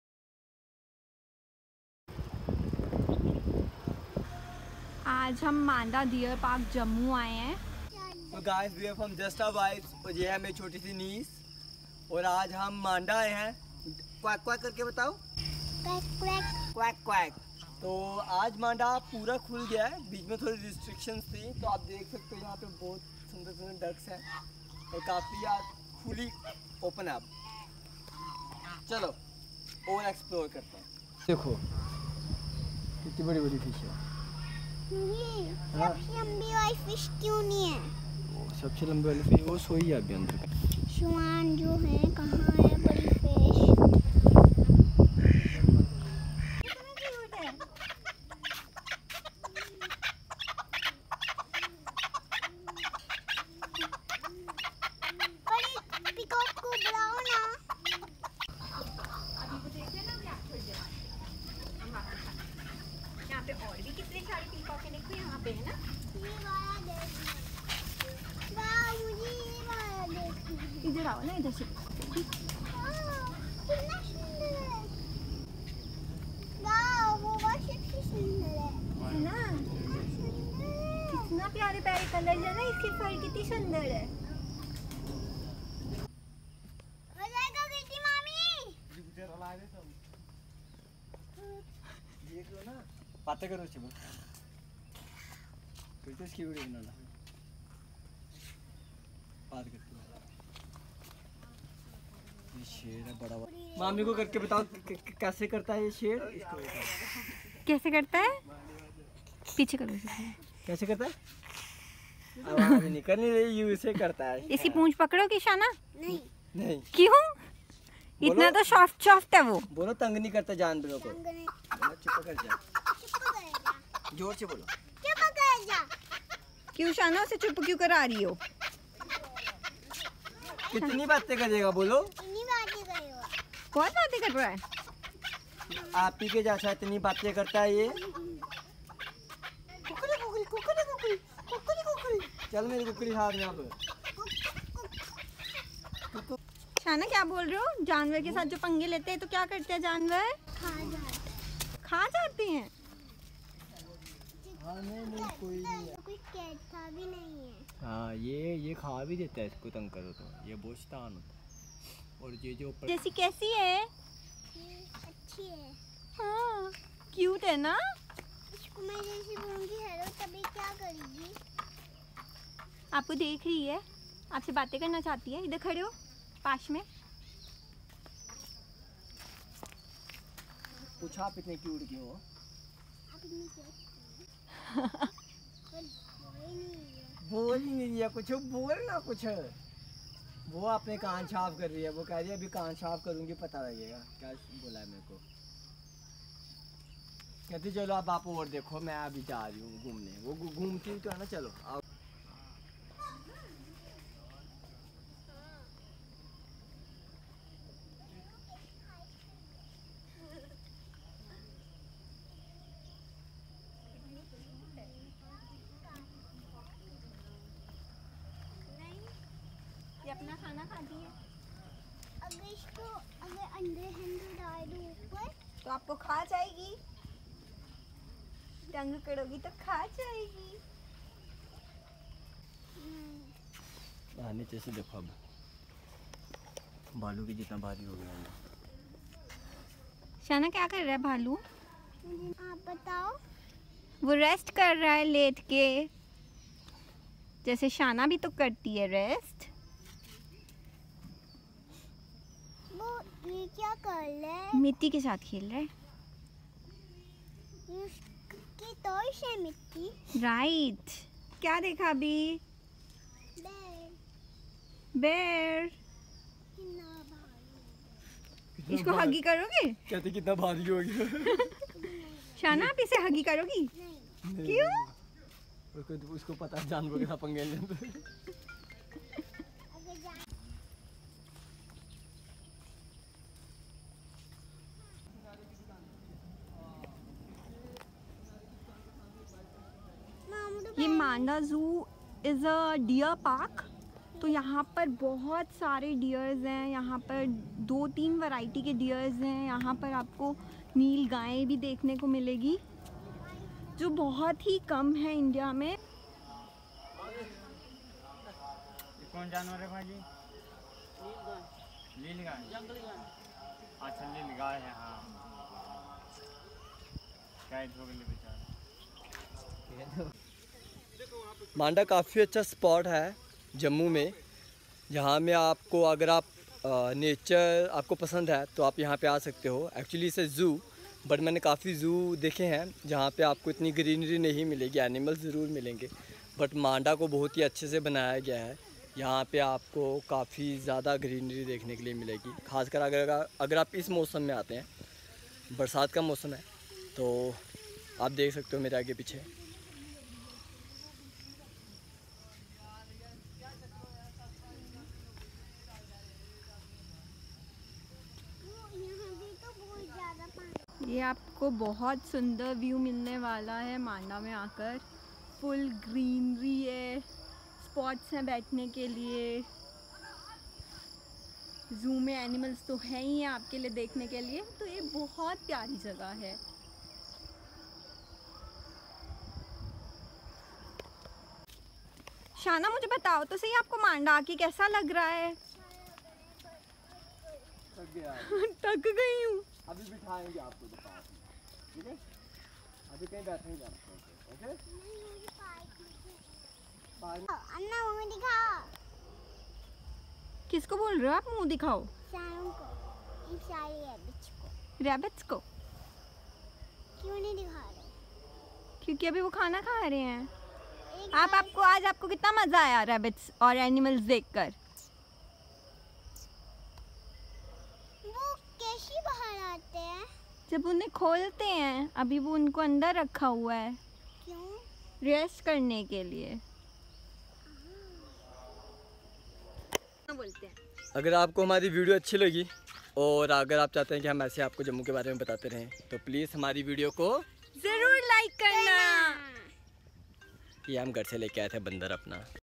Manda Zoo Ka Safar Sheryl Ke Sath - Deer Park (Near Hari Singh Palace) | Summer Picnic Spot | Jammu
A wonderful day out with our loving niece. Manda Zoo is full of wild animals like Leopards, Bear, Peacock, Lion, Deers, Painted stork, Porcupine and many more. One can also see rare Nilgai (Blue Bull) in this lush green park. You can also enjoy variety of trees with their name on it. This is the best attraction for nature lover. It is a perfect place to visit with families and friends on holidays and weekends. Also, it is close to the royal heritage of Jammu and Kashmir, the Hari Niwas Palace.

Tips :
Wear Shoes
Carry water (No cafe is available inside the zoo)